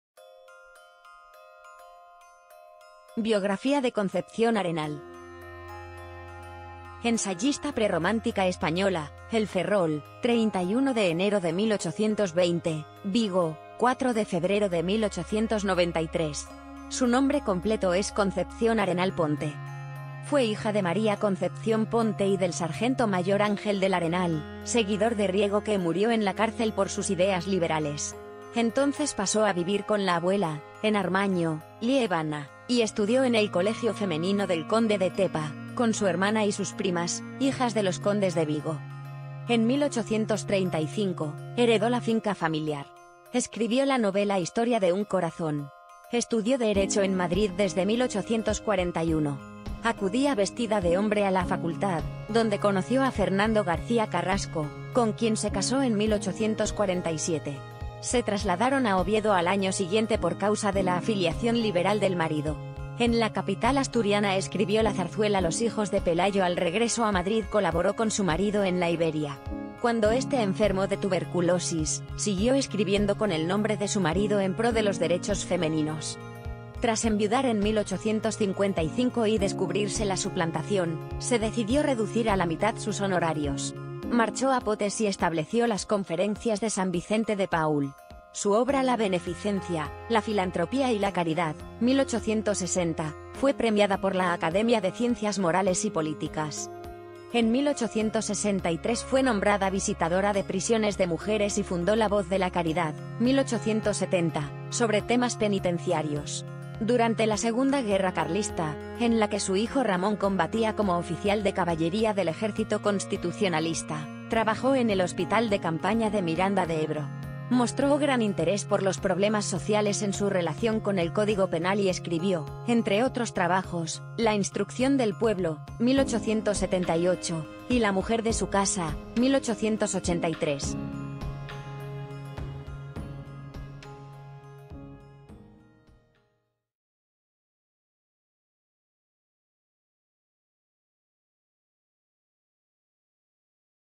Biografía de Concepción Arenal
Ensayista prerromántica española (El Ferrol, 31 de enero de 1820 – Vigo, 4 de febrero de 1893). Su nombre completo es Concepción Arenal Ponte.